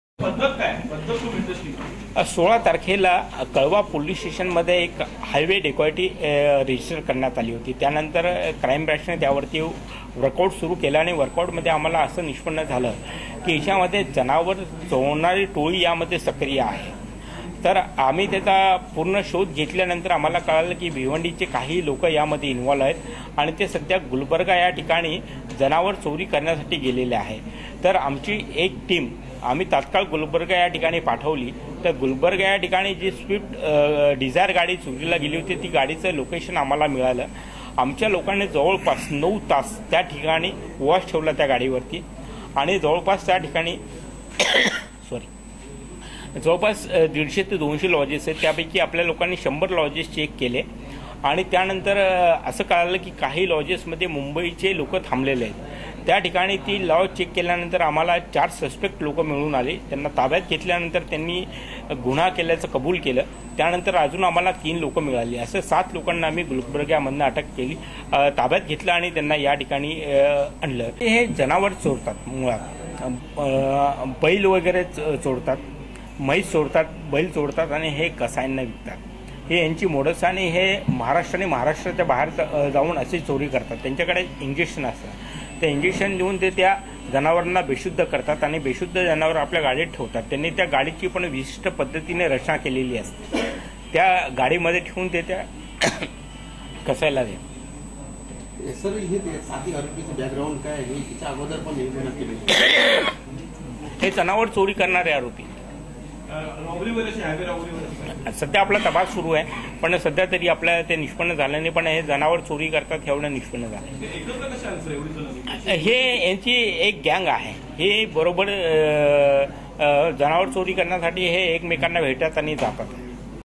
महामार्गावर वाहनचालकास लुटणाऱ्या टोळीला गुलबर्ग्यात अटक, मुद्देमाल जप्त
ठाणे - महामार्गावर दरोडा टाकून वाहनचालकास लुटणाऱ्या ८ जणांच्या टोळीचा छडा लावण्यास ठाणे गुन्हे शाखा युनिट १ च्या पोलिसांना यश आले आहे. या आरोपींना कर्नाटक राज्यातील गुलबर्गा येथून अटक करण्यात आली आहे. आरोपींकडून एक कार, रोख रक्कम, पाच मोबाईल आणि जनावरांना गुंगी देणाऱ्या औषधांच्या 3 बाटल्या व इंजेक्शन असा मुद्देमाल हस्तगत केला आहे. ही टोळी जनावरे चोरुन त्याची कसायाला विक्री करत असल्याचेही उघड झाले आहे.